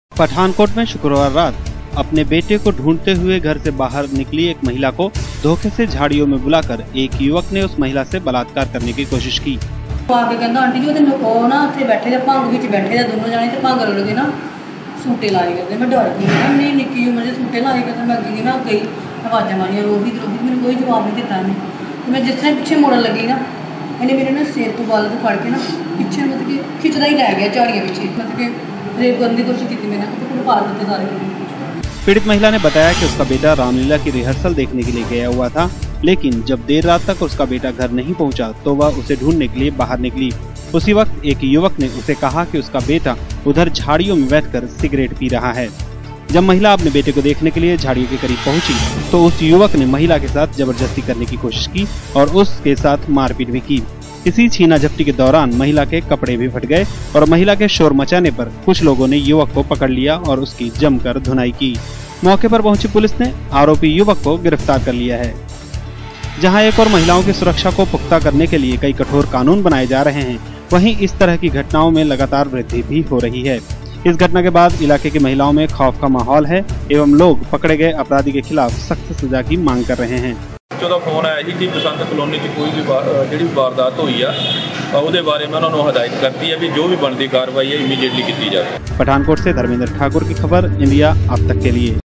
PATHANKOT LADY MISSBEHAVE
पठानकोट में महिला के साथ बदतमीज़ी की कोशिश , मनचले की पिटाई !